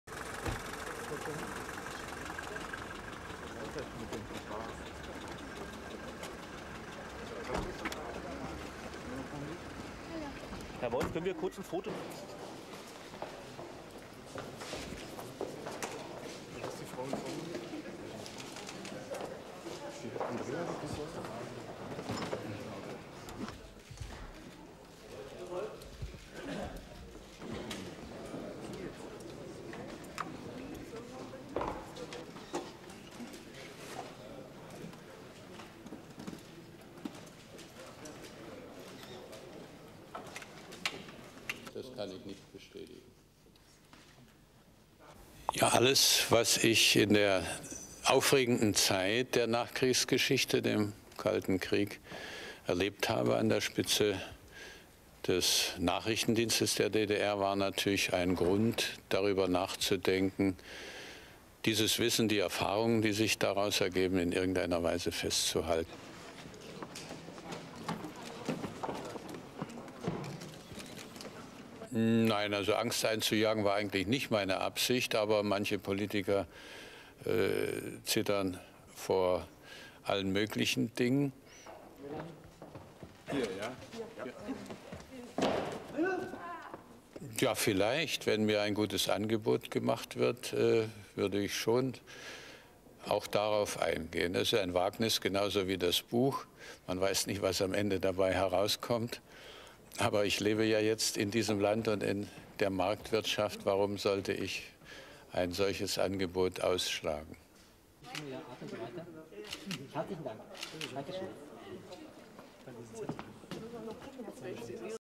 GERMANY: FORMER SPY MARKUS WOLF UNVEILS NEW BOOK
(2 Jun 1997) German/Nat

Former East German spy chief Markus Wolf has traded his shroud of secrecy for a public unveiling with Monday's launch of his memoirs.

The book is called ``Man Without a Face,'' as Wolf was known for years by Western intelligence agencies which could find no picture of him.

Out of the cold and into the hot lights of publicity, former East German spy Markus Wolf arrived at a Berlin theatre on Monday to unveil his memoirs.

The man whose face was unknown to western intelligence agencies for decades was a willing subject for the scrum of photographers taking his picture.

The book, ``Man Without a Face'',  is being published in 13 countries.

In it the 74-year-old Wolf describes his work as head of communist East Germany's espionage network from 1953 to 1986.

He said his unique experience of being privy to so many secrets compelled him to put pen to paper.

SOUNDBITE: (German)
"All the experiences that I had in the exciting time after the
war, the Cold War, in top position of the GDR secret service was a
reason to think about keeping the knowledge and the experience in some way."
SUPER CAPTION: Markus Wolf, former East German spy

Wolf said he assumed moral guilt for his past history of spying but there was nothing new in his book to embarrass others.

SOUNDBITE: (German)
"No, to frighten someone was not my intention but some politicians
tremble because of different things."

Wolf was asked if he'd like to see his book made into a movie.

SOUNDBITE: (German)
"Yes maybe if someone makes me a good offer I would do it. It is a risk like the book you never know the result of it. But since I live in this country facing market economy why should I refuse an offer like that."

The former communist who was once camera-shy seems as comfortable in a capitalist system as he is with the spotlight.